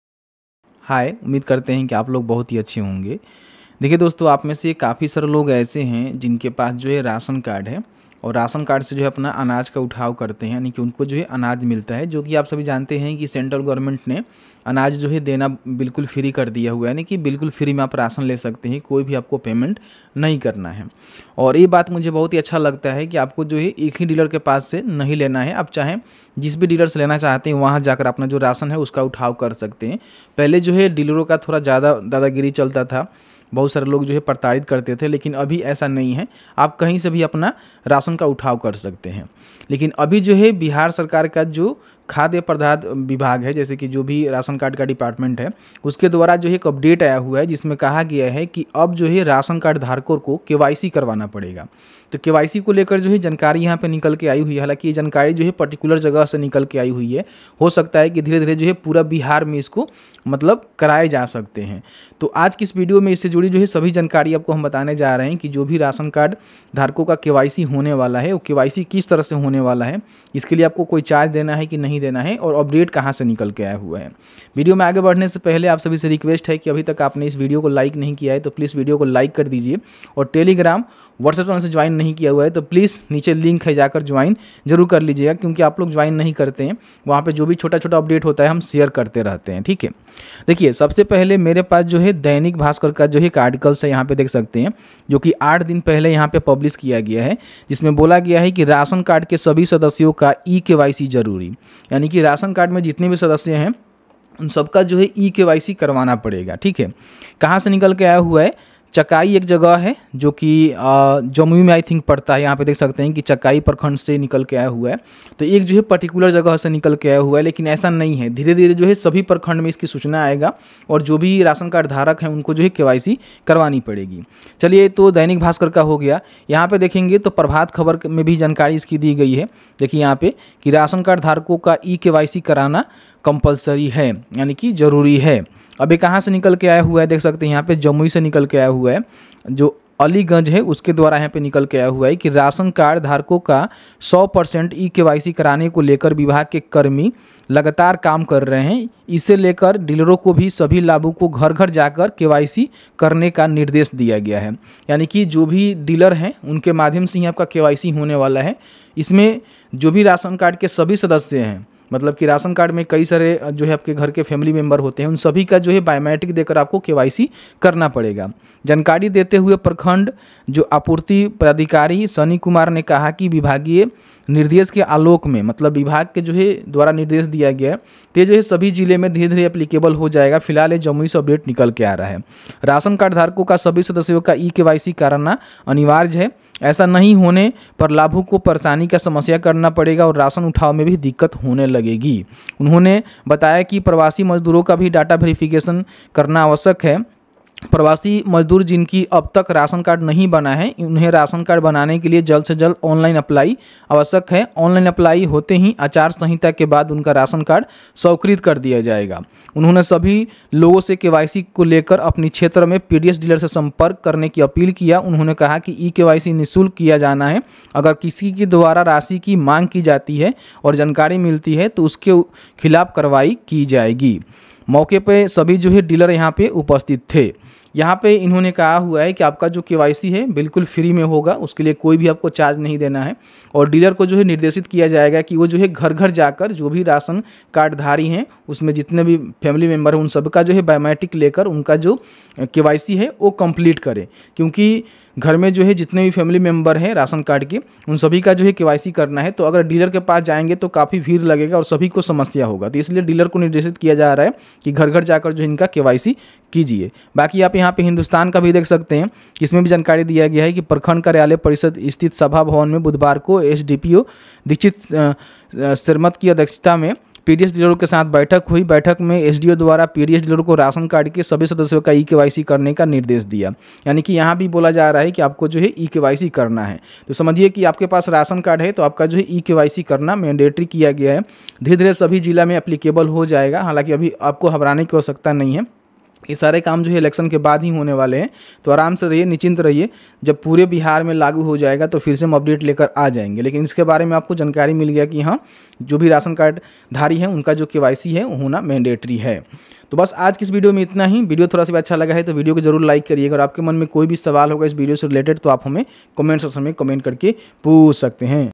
Bihar Ration Card Kyc Big Update | अब सभी राशन कार्ड धारियों को करना होगा kyc वरना राशन कार्ड बंद
Bihar Ration Card Big Update | अब सभी राशन कार्ड धारियों को करना होगा kyc वरना राशन कार्ड बंद